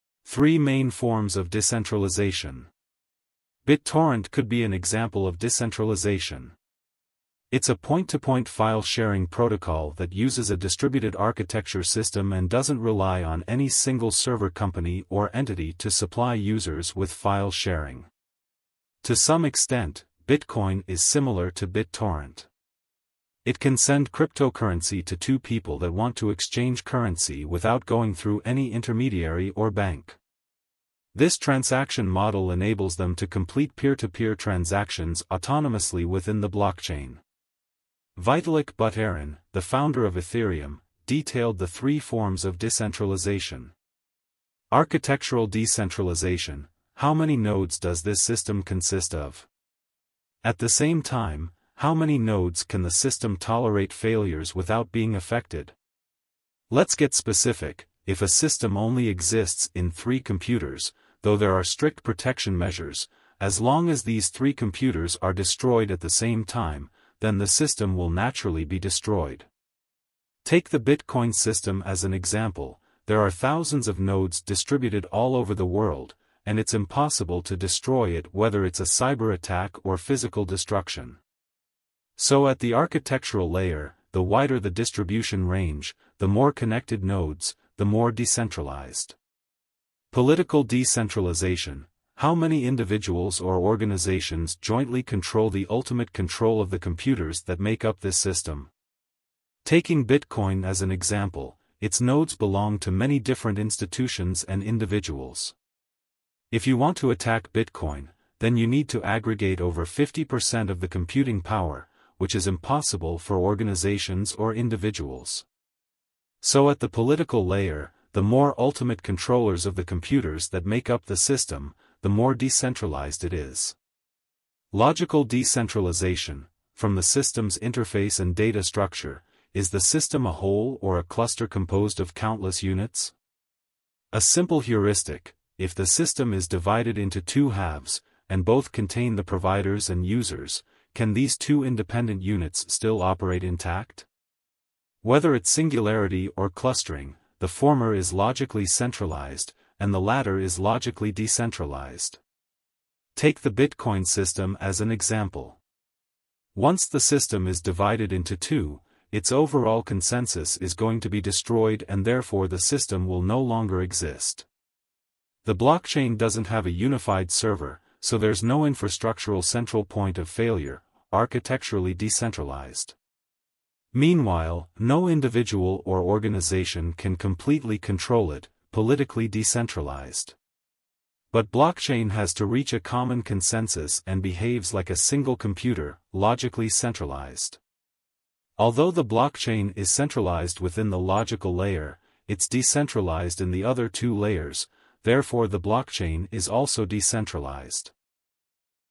Unveiling Decentralization's Architectural, Political, and Logical Aspects
Blockchain is logically centralized, it excels in architectural and political decentralization, making it a robust decentralized system.